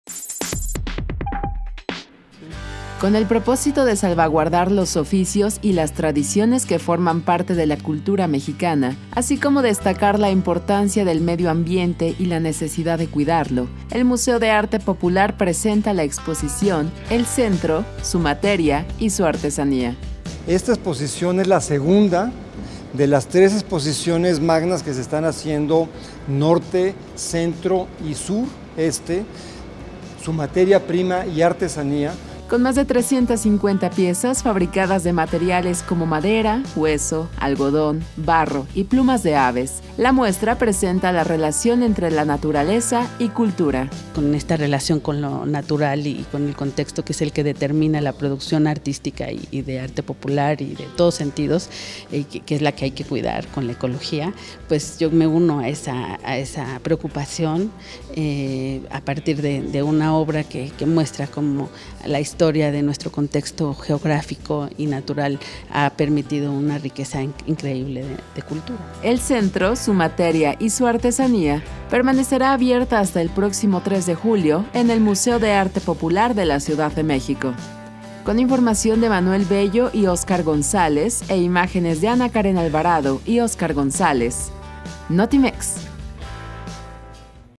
Museo de Arte Popular exhibirá relación entre artesanía y biodiversidad
Más de 350 piezas que muestran la relación entre la producción de artesanías y la biodiversidad, conforma la exposición "El Centro, su materia y su artesanía", que se presenta a partir del 2 de abril